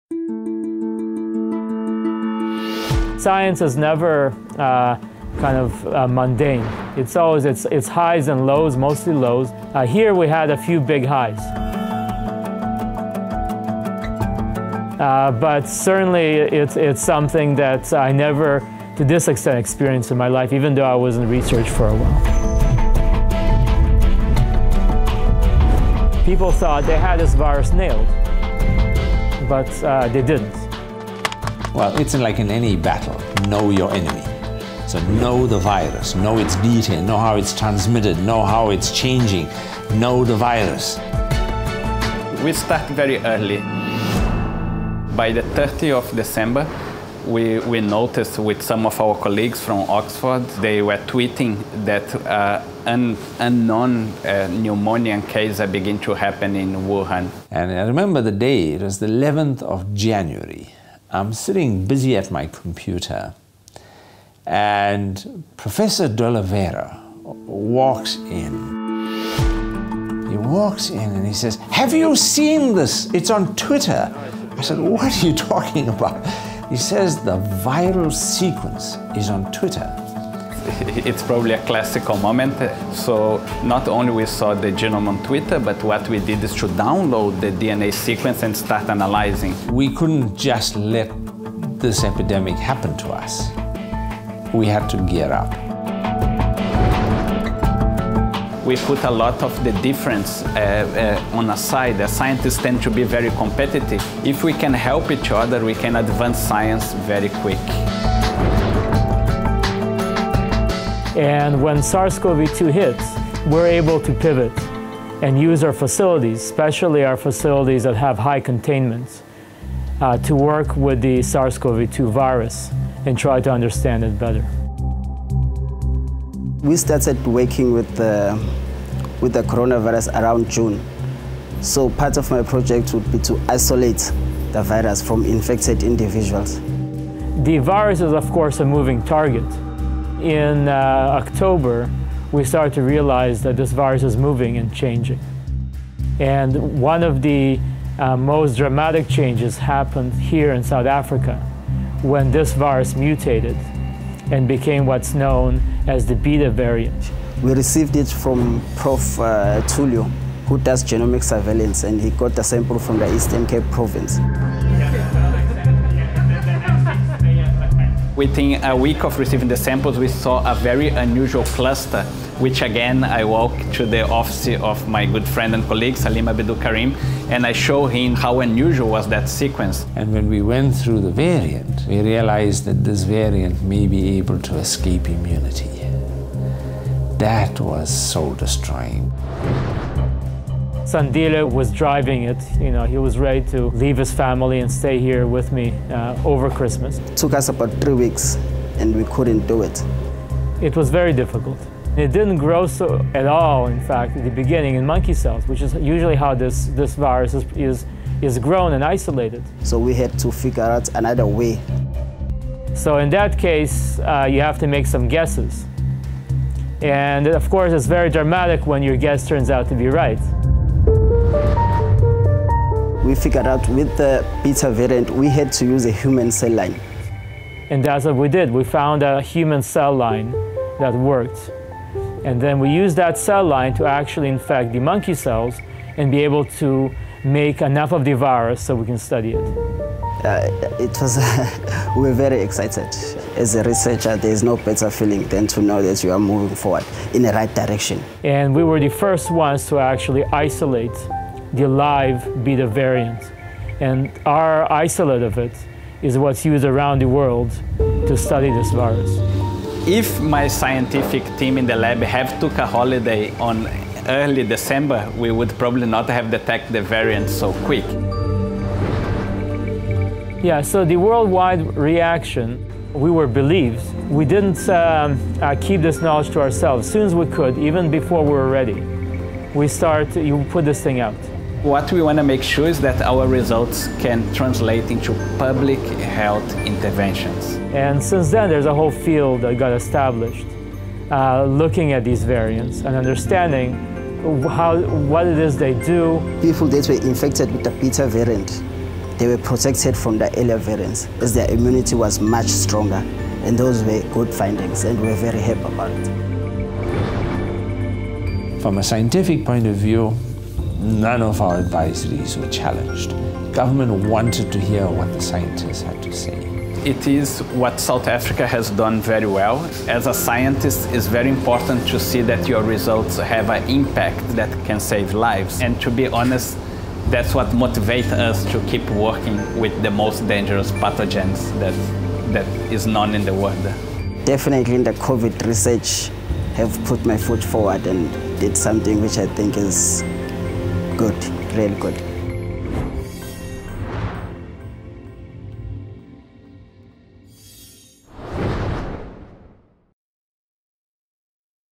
SA’s scientists deliver huge boost to global COVID-19 fight
Scientific collaboration through PeopleWithPurpose led to a world-first for South Africa in January 2021. Scientists isolated the Beta variant of COVID-19 after doctors in the Eastern Cape reported an unusual spike in infections